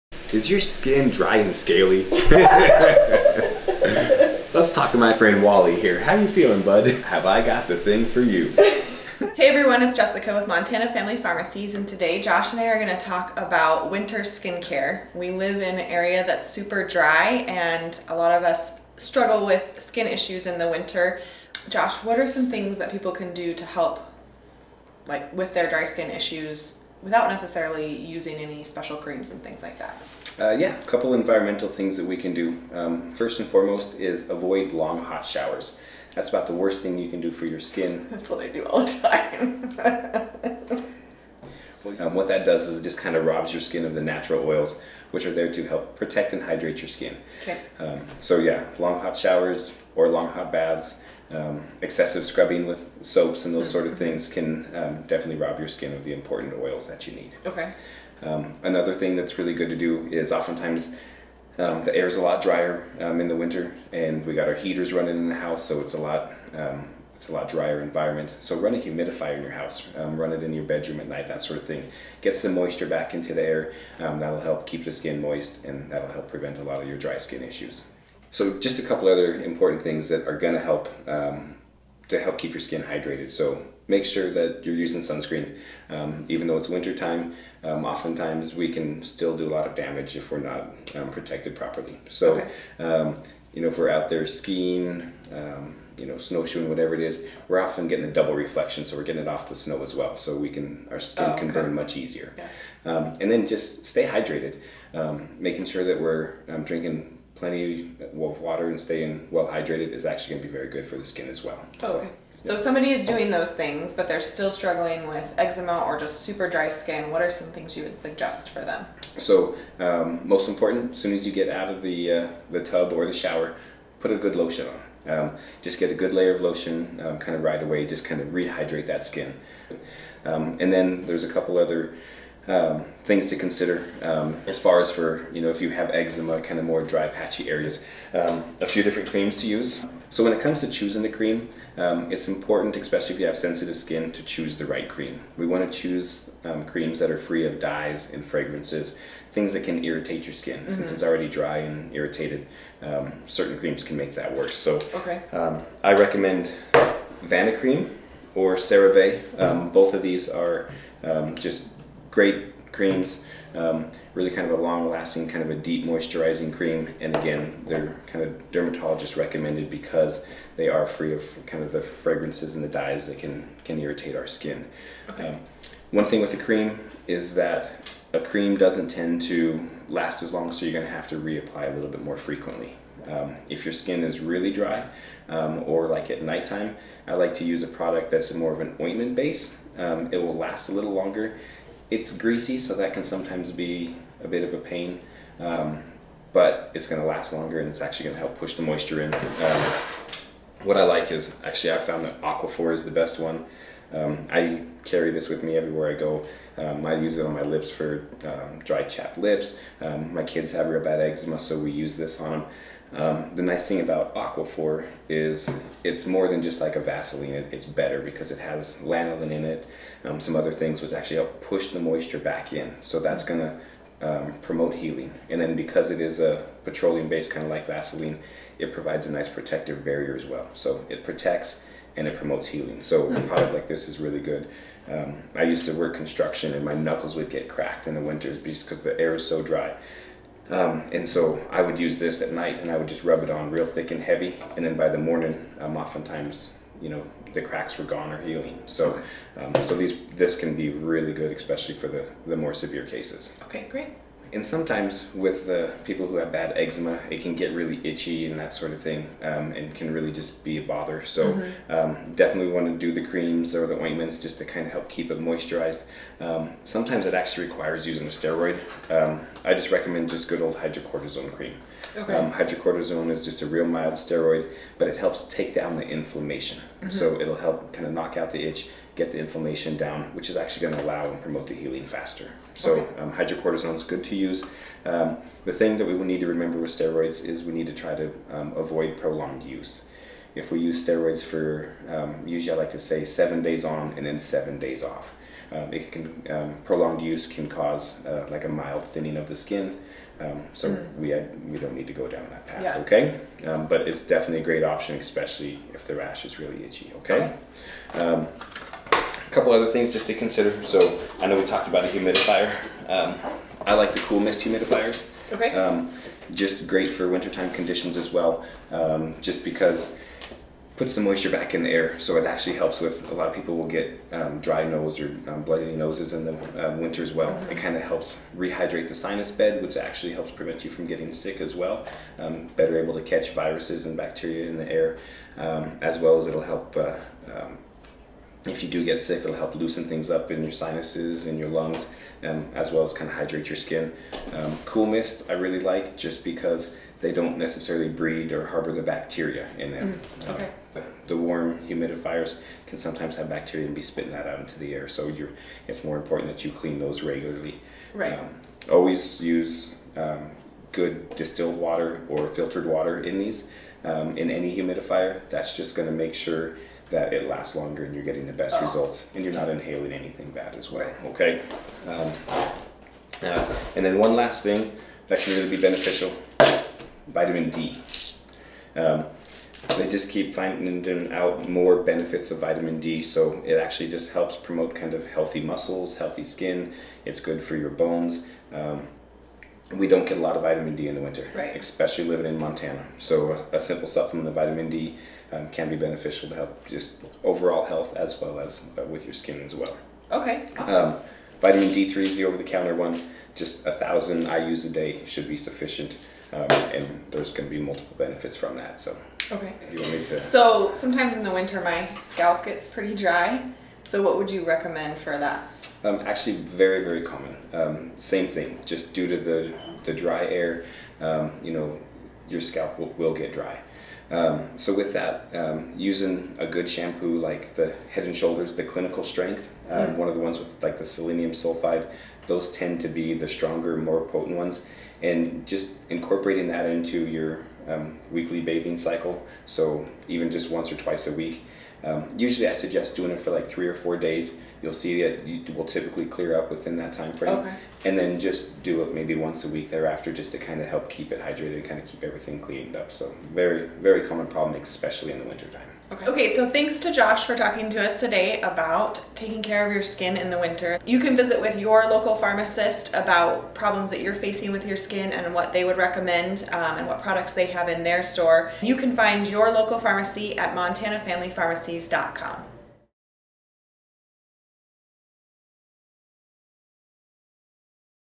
How to Take Care of Your Skin in the Dry Winter Months
Skin issues tend to flare up in dry winter months, especially in northern climates like ours here in Montana.

Whether it's avoiding hot showers or if your skin needs the occasional application of hydrocortisone cream, learn what you can do to help keep your skin healthy and itch-free this winter from Pharmacist Josh Fisher.

In this video, you'll learn:
  • How you can help keep your skin as moisturized as possible without using any special creams or ointments (0:21)
  • How you can help soothe dry, sensitive skin using non-medicated, fragrance-free creams (1:52)
  • What you can use to treat very dry skin without having to constantly reapply (3:05)
  • When itchiness and eczema call for hydrocortisone cream and how often to use it (4:14)
  • The vitamin that may be beneficial to your skin when taken consistently (among other benefits!) (6:13)
  • What you can do to treat your dry, scaly scalp (6:49)

Your local pharmacy will be glad to help you with your own winter skin care concerns and questions. Find your local Montana Family Pharmacies Member pharmacy